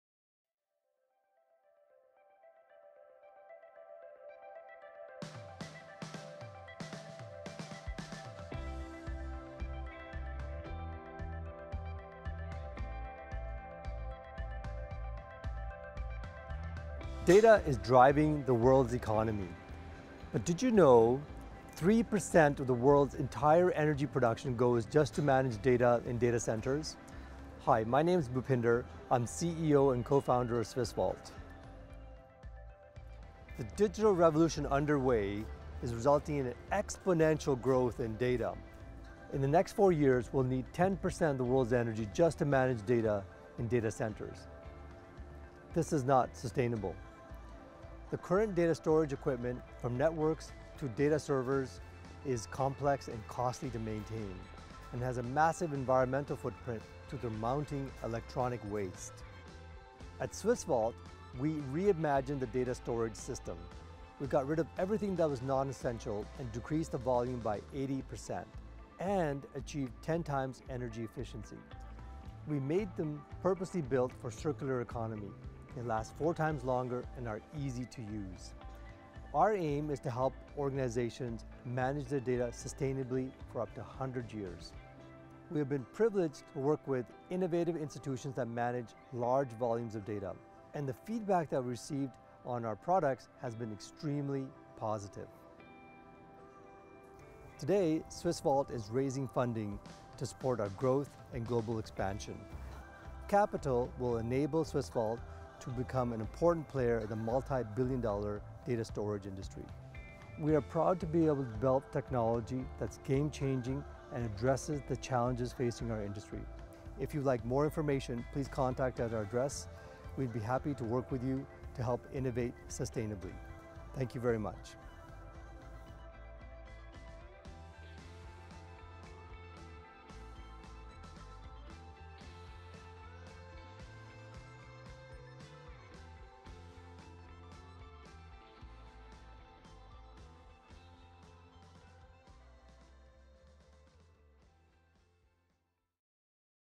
Swiss Vault | Green Tech Investor Pitch | Sustainable data storage solution
Swiss Vault re-imagined the data storage system, getting rid of all the unnecessary components. By doing so, Swiss Vault’s solution represents a new paradigm through revolutionary hardware designs and reimagined software integration. The patent pending data management systems reduces the size by 80%, and achieves 10 times more power efficiency, compared to traditional systems. Swiss Vault’s systems are also purposely built for the circular economy: they are easy to operate and last 4 times longer than rack systems. Swiss Vault aims at enabling organizations to manage their data for 100 years, with lower costs and less impact on the environment.